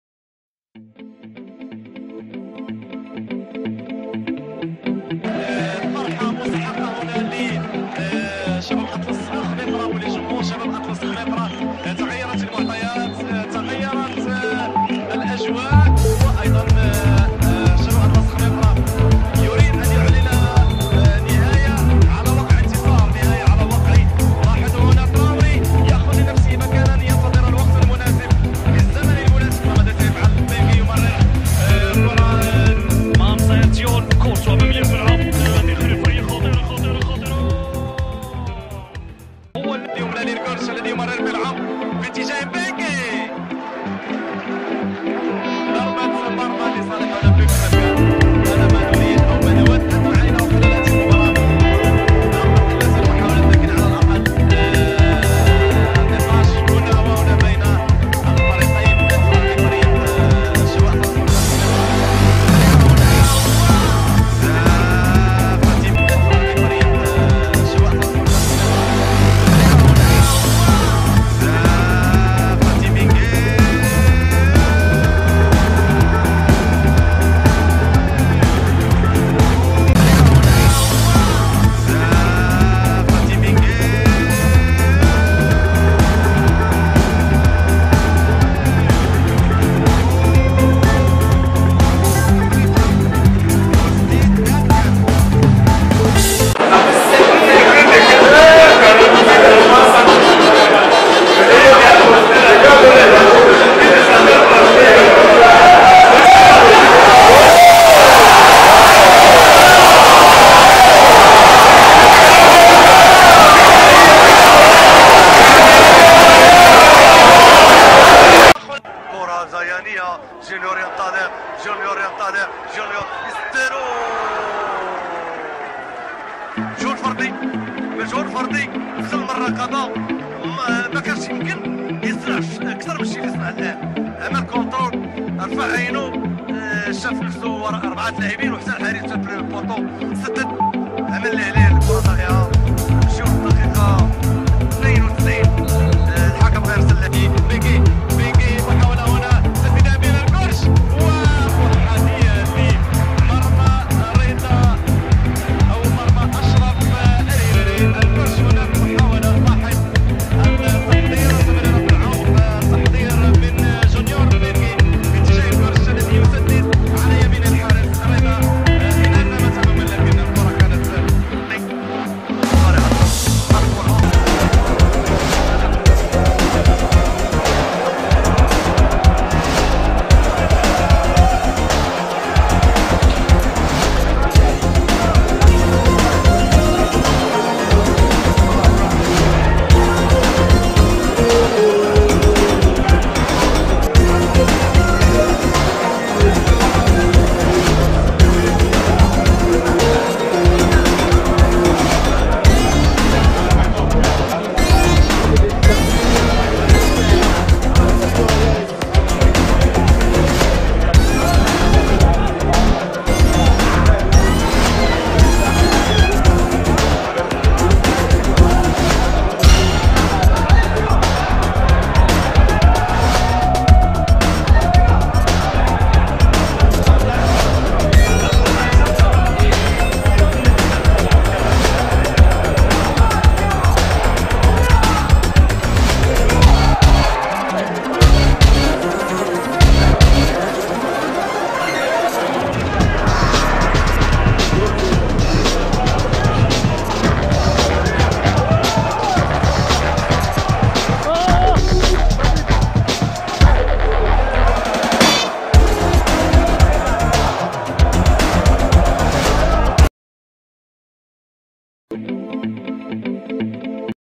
Junior ATEMENGUE - ATTAQUANT 1M95
Jeune avant-centre camerounais en pleine progression :  1m95 de puissance, de technique et d'intelligence footballistique